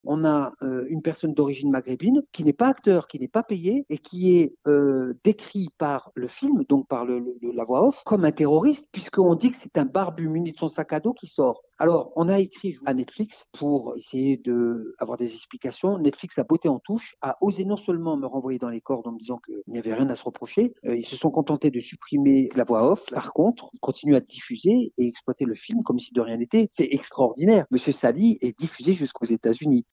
Filmé à son insu par Netflix, ce Niçois porte plainte contre le géant américain
Un jeune niçois attaque le plateforme de streaming Netflix pour s'être retrouvé dans un film sans son consentement. Son avocat, Me Jean-Pascal Padovani a également porté plainte pour provocation publique à la discrimination et à la haine raciale